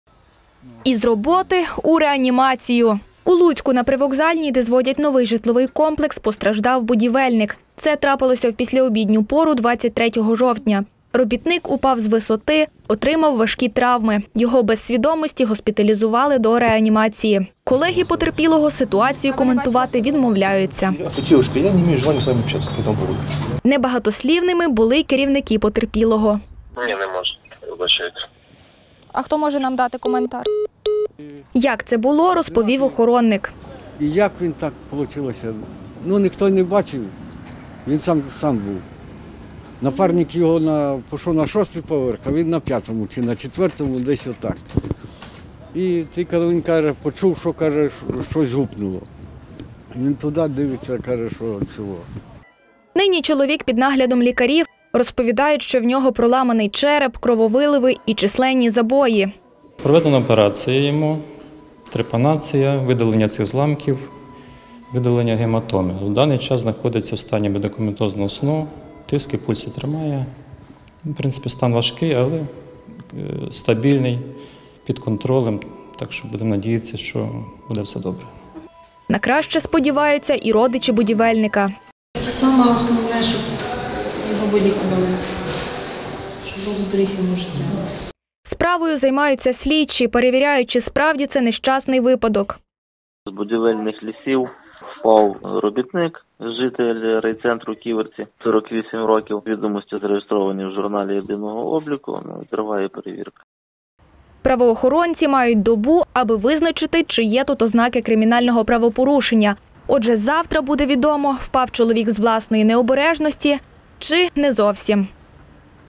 Падіння будівельника: чоловік у важкому стані
У Луцьку, у вівторок, 23 жовтня, в післяобідню пору, на будівництві постраждав чоловік. Він впав з висоти і отримав численні травми. Нині потерпілий - у реанімації. Досі не опритомнів, - кажуть у приймальному відділені. На місці події працюють правоохоронці.